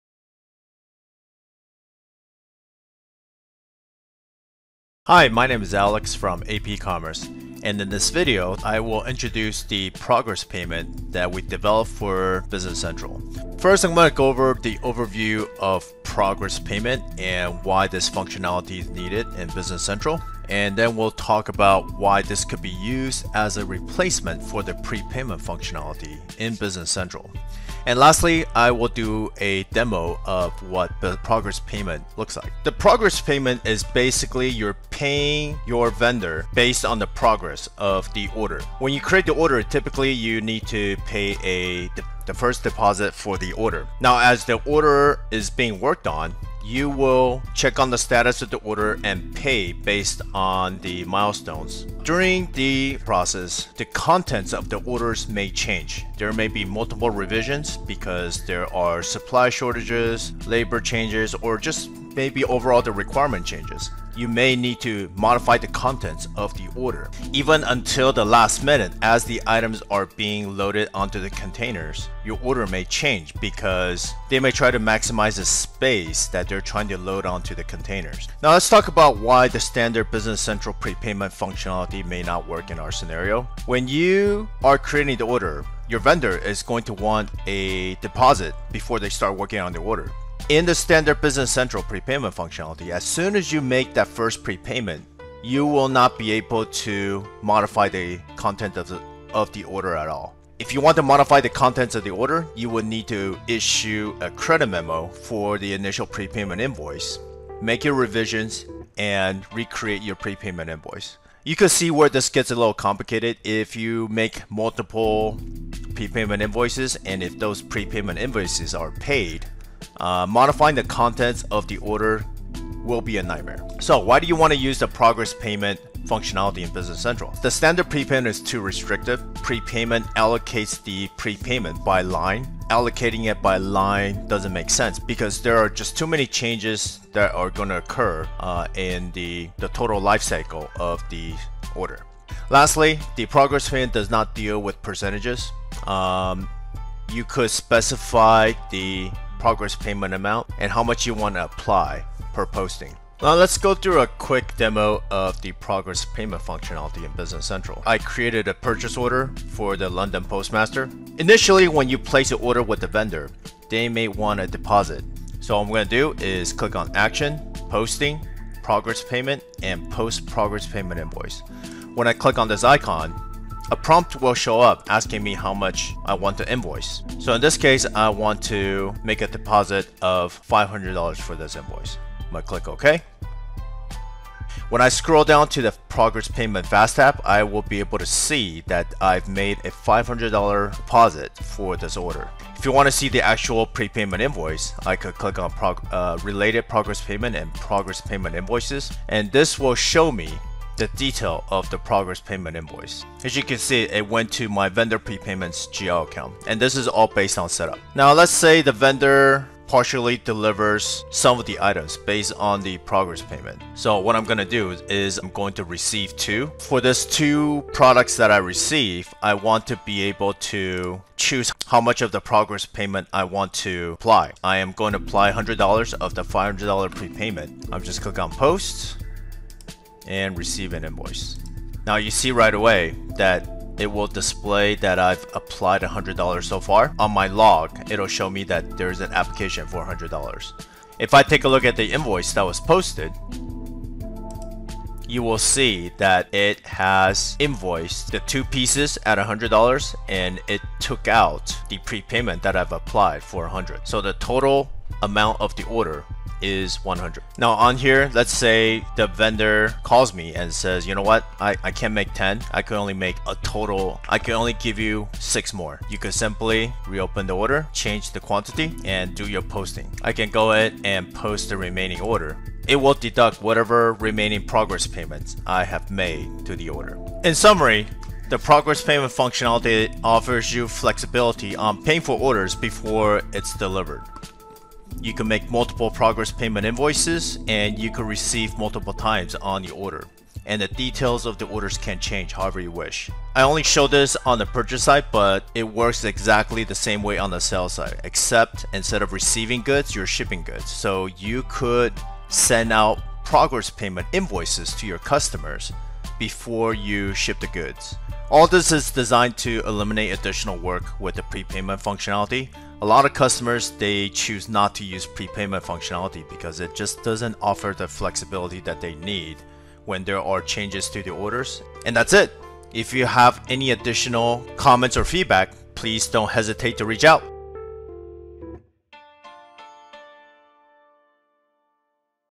Progress Payment for Dynamics 365 Business Central
Use progress payment functionality for Dynamics 365 Business Central to prepay for sales and purchase orders on the order level instead of allocating it on the line level.

This allows you to have flexibility to change the details of the order even when if the order is prepaid without having to do a credit and re-invoice.